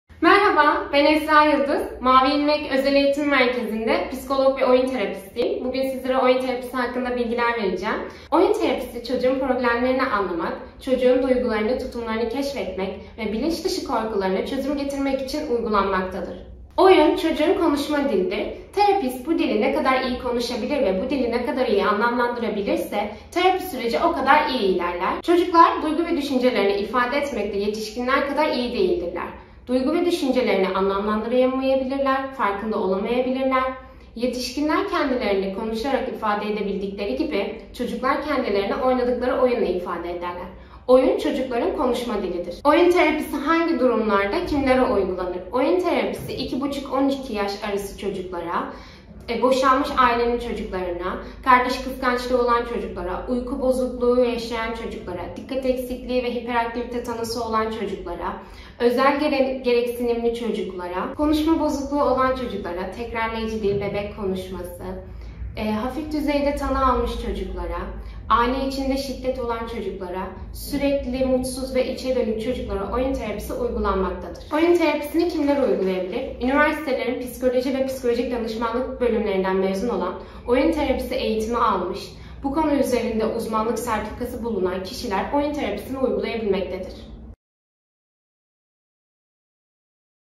Oyun Terapisi
Kurumumuz Oyun Terapisti ve Psikologlarından Esra YİLDİZ, Oyun Terapisi Hakkında Bilgiler Verdi.